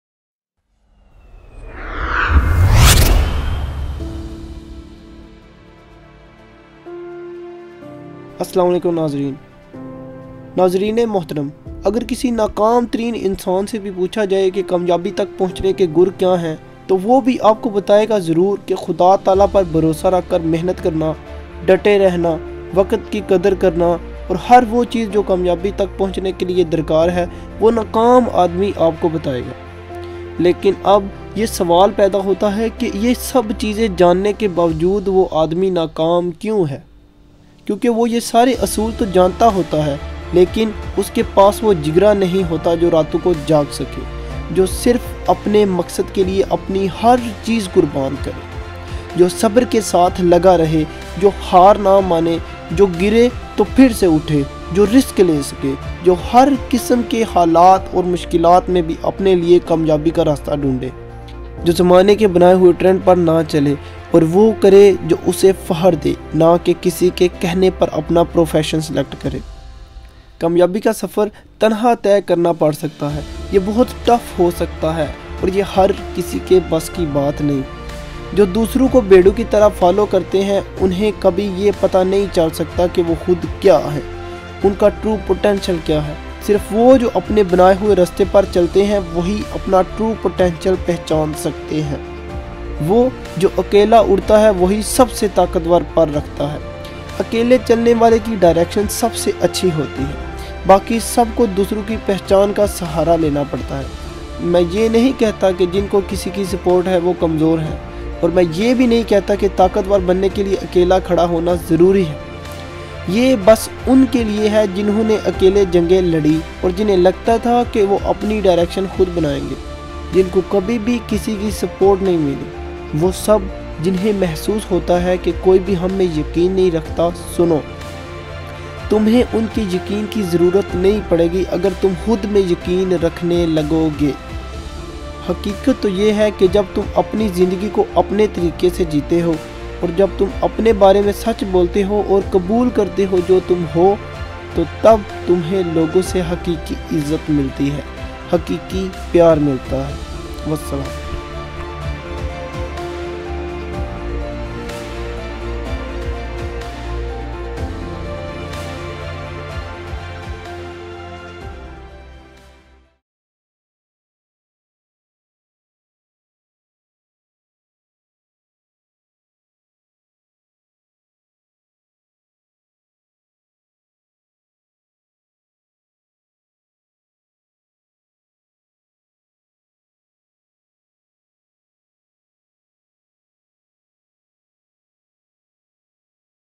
Believe Yourself. Bharosa khud par .A motivational video by UMAIR ALAM
HAR KOI KAMYABI KA RASTA JANTA HA par us rasty par aany wali mushkilat ki waja sy wo kuch b ni kar sakta

is video me is mozu par baat ki gayi ha

UMAIR ALAM

You can also check our page at facebook